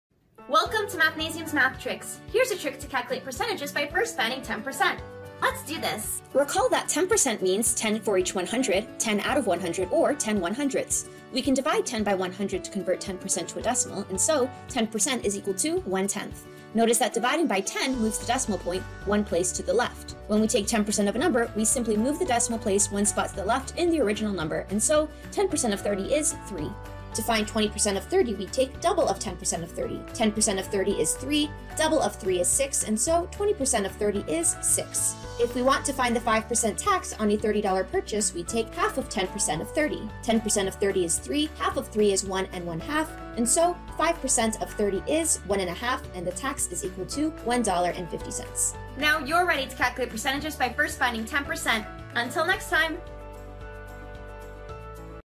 Mathnasium MathTricks | Percentages Part 4
:56 Live Mathnasium MathTricks Demo on calculating percentages by first finding 10%.